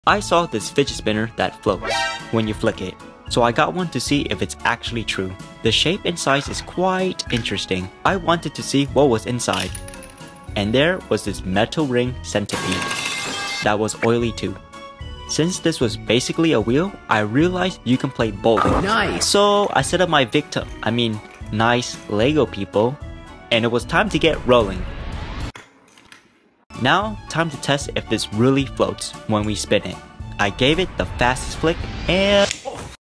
Will this fidget spinner float? 🤔
This is the QiYi Haptic Coin spinner! They made a video that it floats let’s see if it’s actually true!